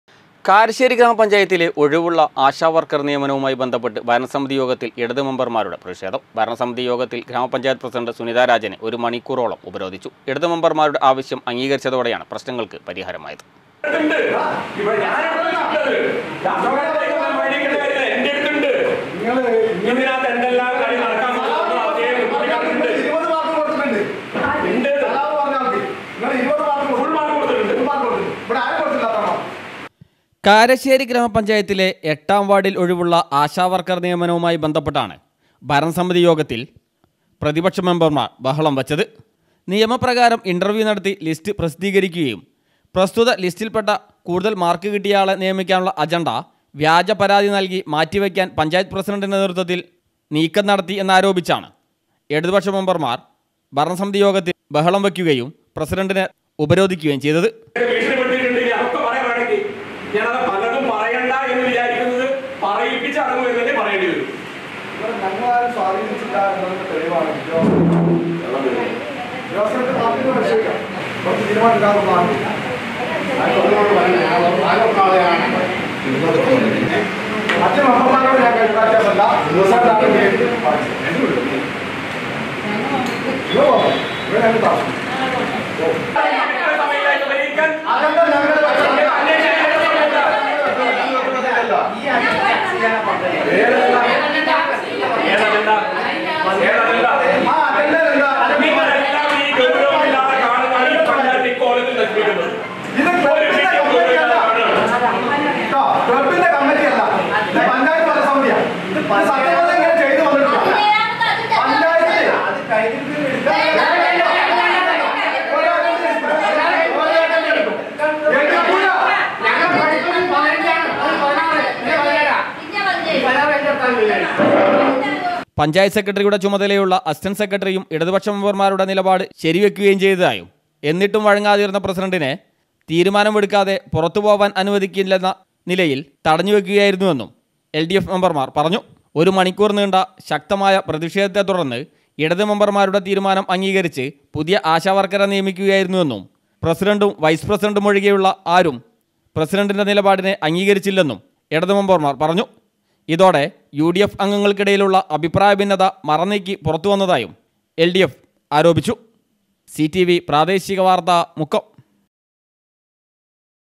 .ഭരണ സമിതി യോഗത്തിൽ ഗ്രാമ പഞ്ചായത്ത് പ്രസിഡൻ്റ് സുനിത രാജനെ ഒരു മണിക്കൂറോളം ഉപരോധിച്ചു.
കാരശ്ശേരി ഗ്രാമപഞ്ചായത്തിലെ ഒഴിവുള്ള ആശാവർക്കർ നിയമനവുമായി ബന്ധപ്പെട്ട് ഭരണസമിതി യോഗത്തിൽ ഇടത് മെമ്പർമാരുടെ പ്രതിഷേധം.ഭരണ സമിതി യോഗത്തിൽ ഗ്രാമ പഞ്ചായത്ത് പ്രസിഡൻ്റ് സുനിത രാജനെ ഒരു മണിക്കൂറോളം ഉപരോധിച്ചു.